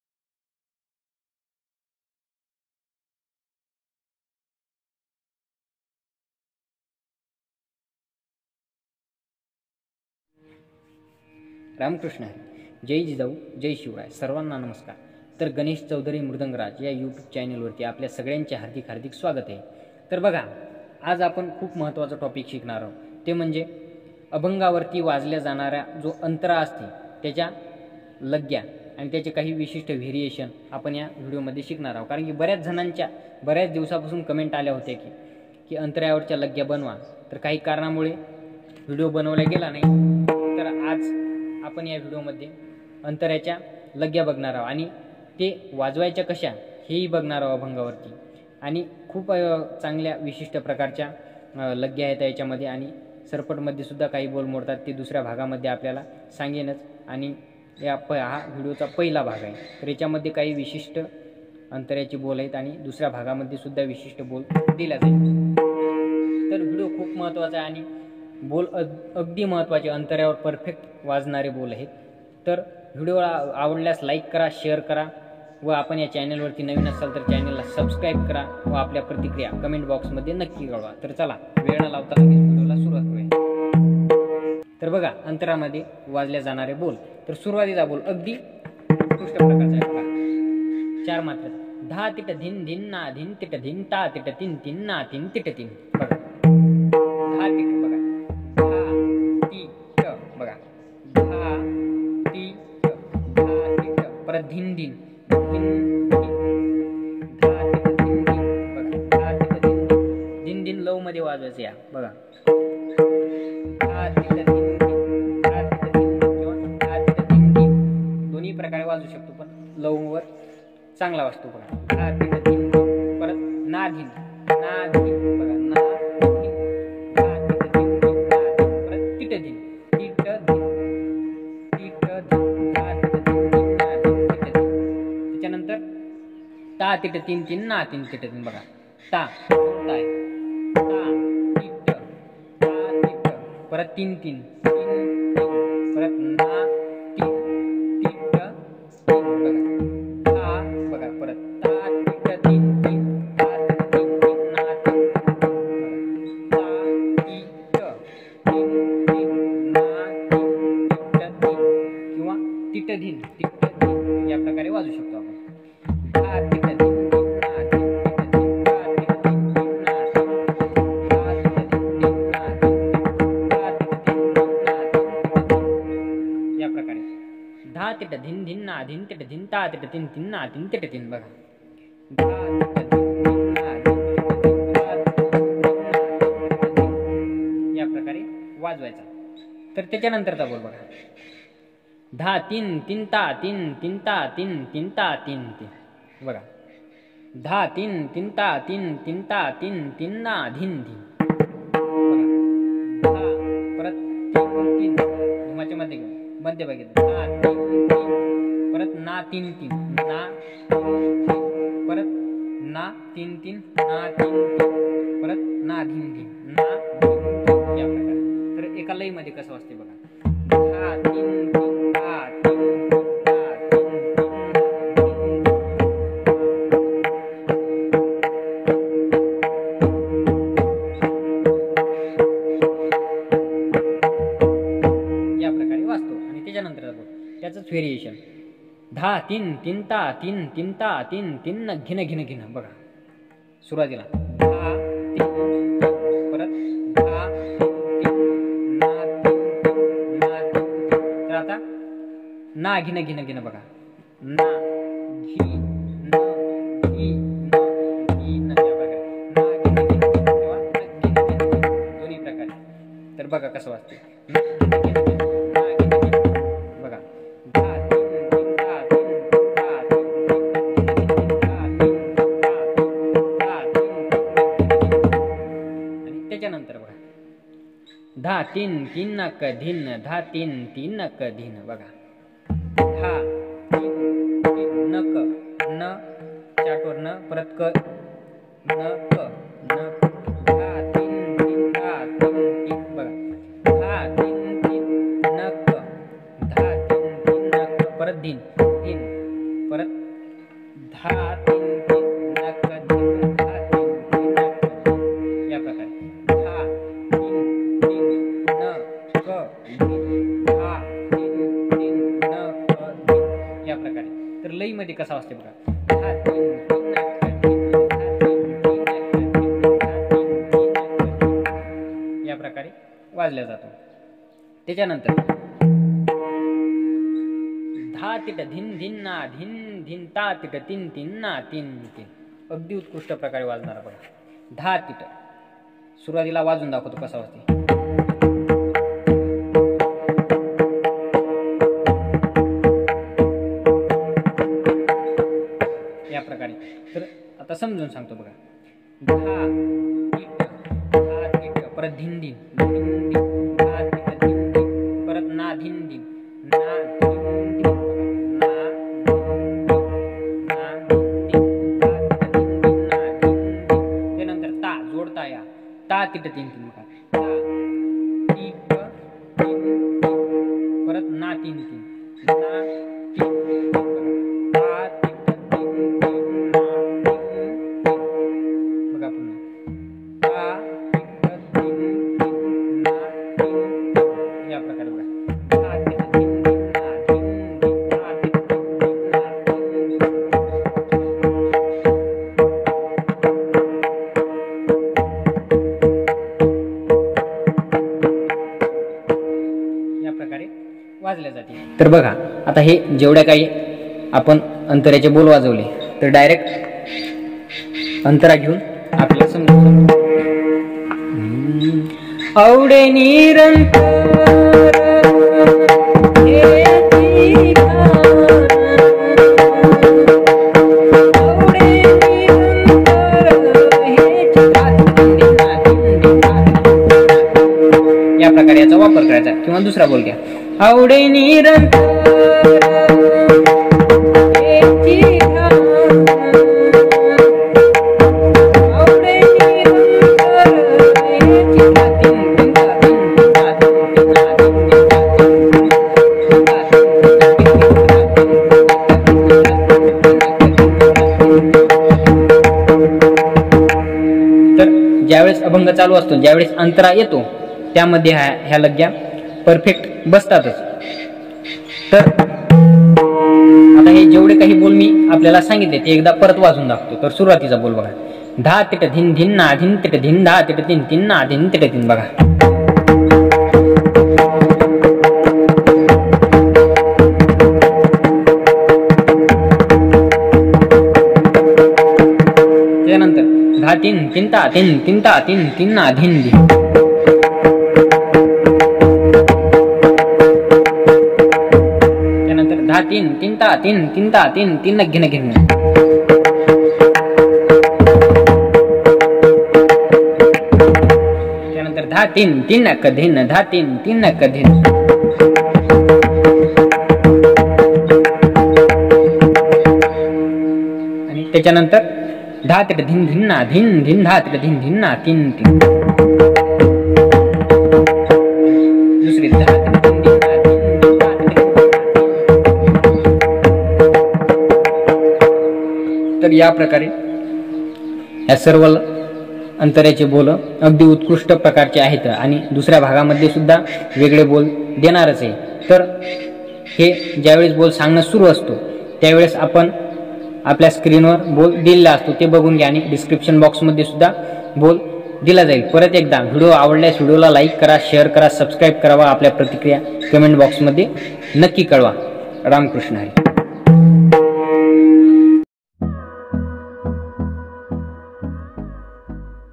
pakhavaj lesson भाग - १/ अंताऱ्या चे बोल / लग्गी ,पखवाज. गणेश म चौधरी चिंचखेडा (आळंदीकर)
१. धातिटधिंधिं नाधिंतिटधिं तातिटतिंतिं नातिंतिटति

२.धातिंतिं तातिंतिं तातिंतिं नाधिंधिं

३. धातिट धिंधिं नाधिंधिं धातिट  धिंधिं नातिंतिं

४ .धातिंतिं तातिंतिं तातिंतिं नाघिनघिनघिन